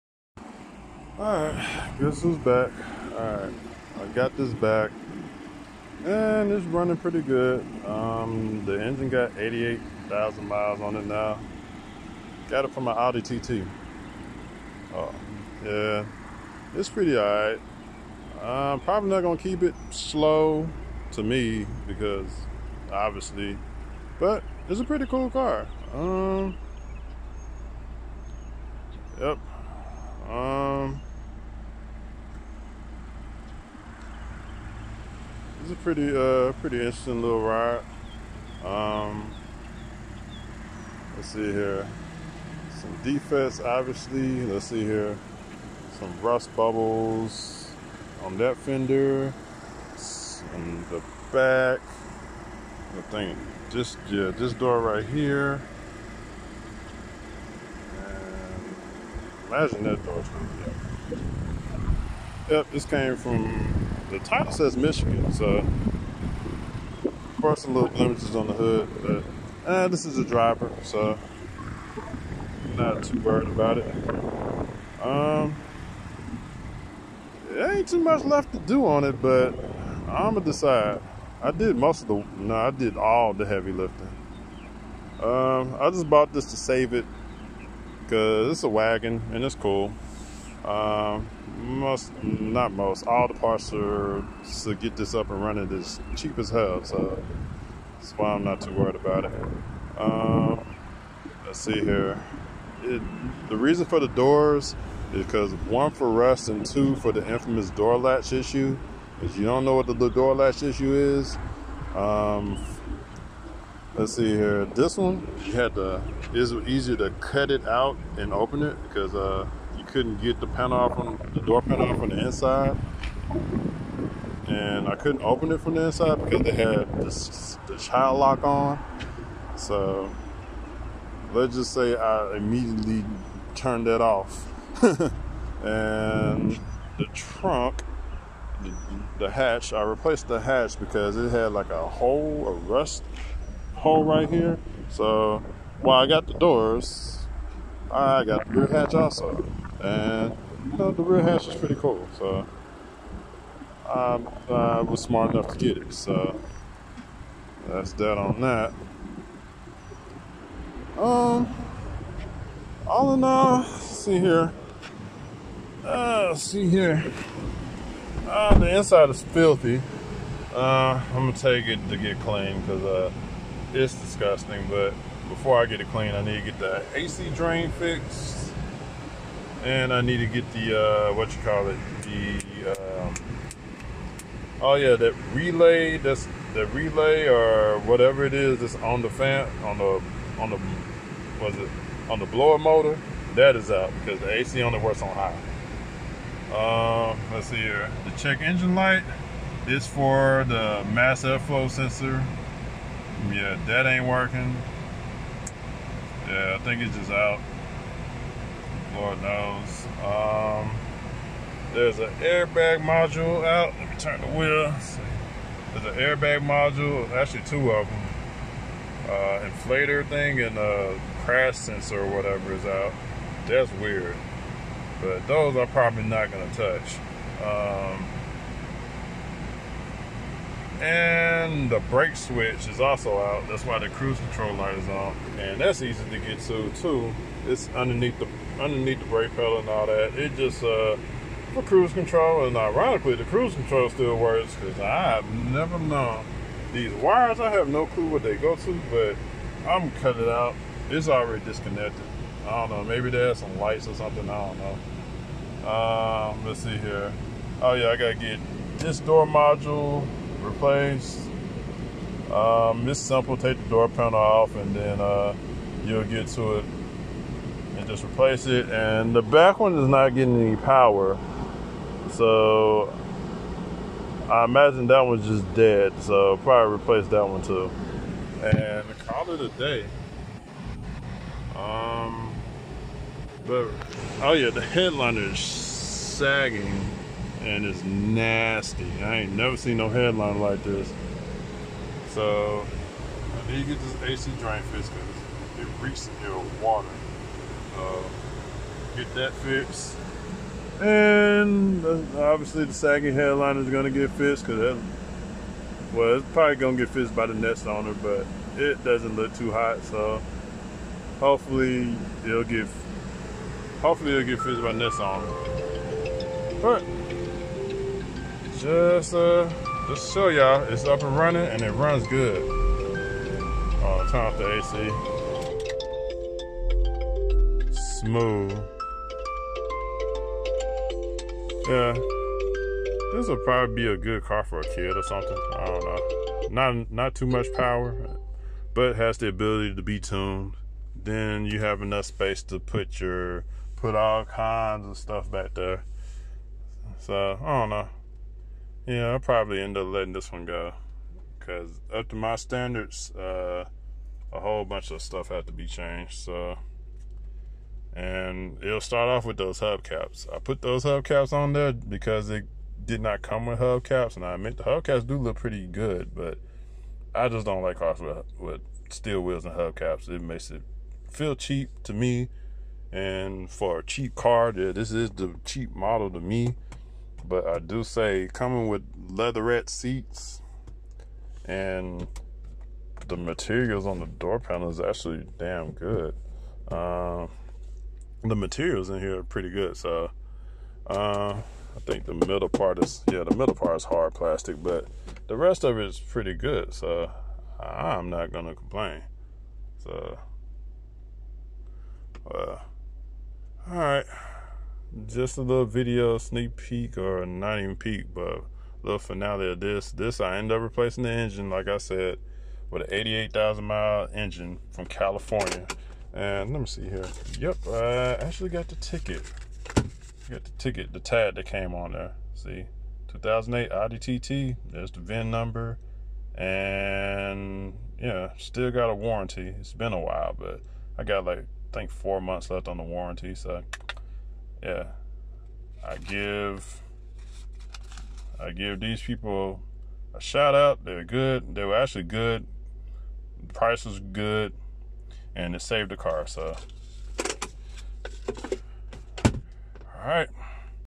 The $600 Dollar Project B6 Passat Wagon is Back!
The new engine and several major maintenance items was done on the Passat. It still has a few things to get done.